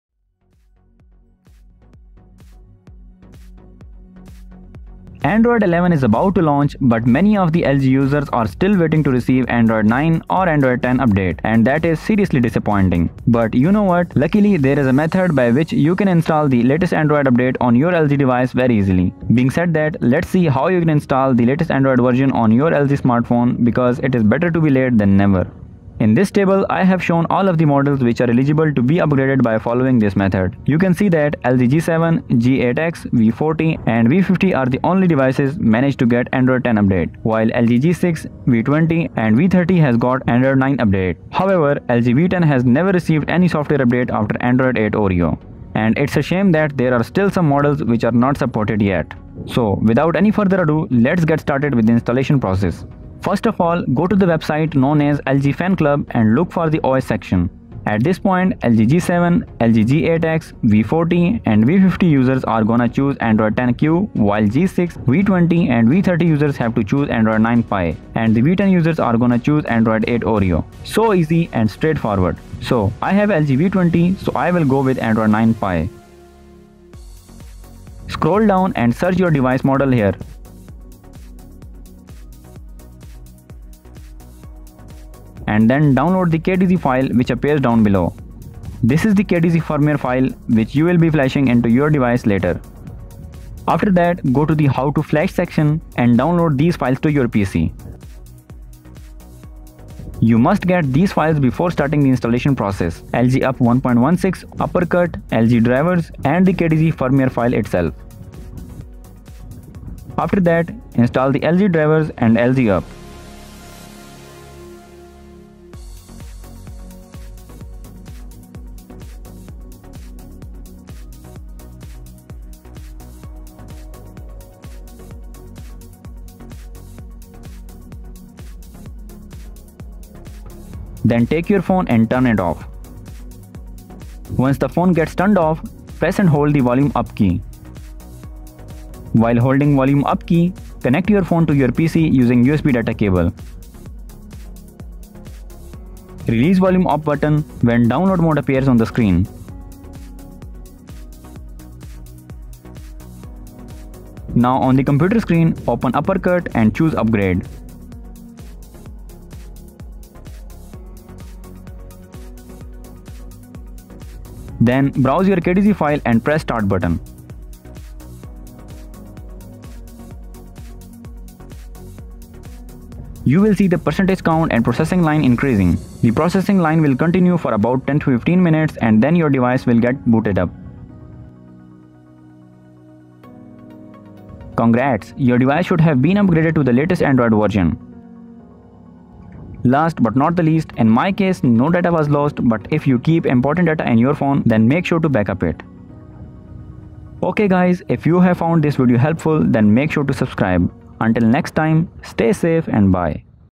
LG G6 / G7 / G8X / V10 / V20 / V30 / V40 / V50 Official Android 10 and 9 Update | Easy Install Guide
I have used LG V20 for demonstration purpose. You can full the exact same method to install latest available Android version for your LG Smartphone. This method works on LG G6, LG G7 ThinQ, LG G8X, LG V20, LG V30, LG V40 and LG V50.

Although the Firmware is official one but sometimes some devices gets stuck in the boot loop. To fix Boot loop problem the only solution would be to Factory Resetting the phone. So, it is recommended to do a backup of your important data just in case.

This helpful video is based on the knowledge/methods learnt online. KHANISTEK has just shown you what worked for him.

Since, he is not the owner of the files so any changes made to the files (after the video is published) has nothing to do with the him. Lastly, Install it on your own risk, coz there is a slight chance of bricking the device.